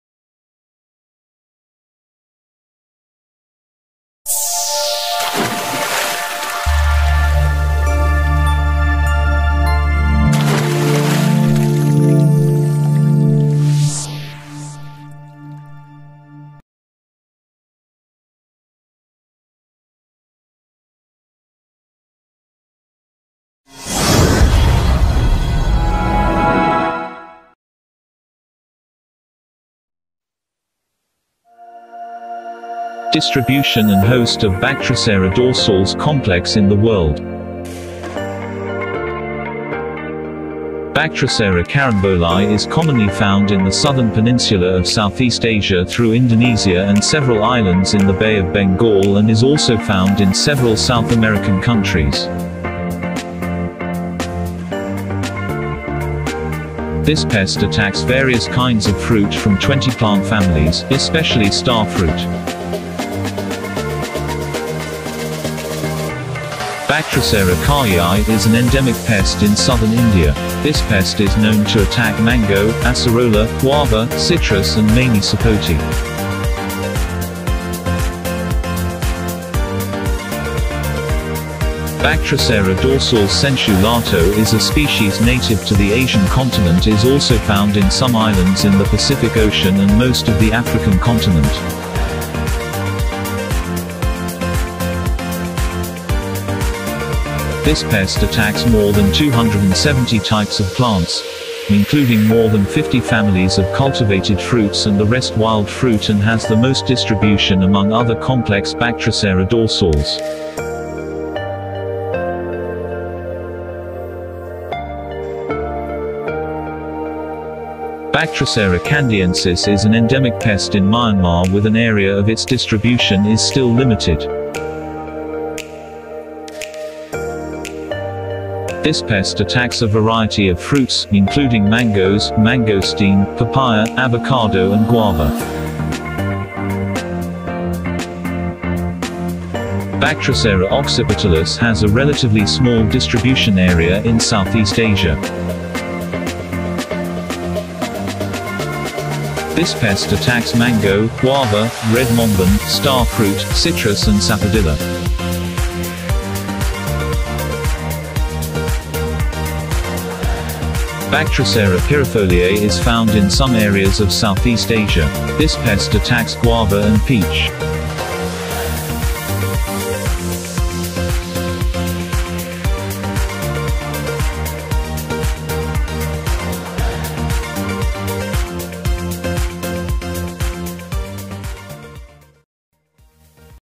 Distribution and Host of Bactrocera dorsalis Complex in The World
ISPM 27 Protocol Diagnostic 29: Bactrocera dorsalis.

B. carambolae is commonly found in the southern peninsula of Southeast Asia through Indonesia and several islands in the Bay of Bengal (Drew and Romig, 2013) and is also found in several South American countries (CABI, n.d.). This pest attacks various kinds of fruit from 20 plant families, especially star fruit (Averrhoa carambolae) (CABI, n.d.). This pest attacks various kinds of fruit from 20 plant families, especially star fruit (Averrhoa carambolae) (CABI, n.d.).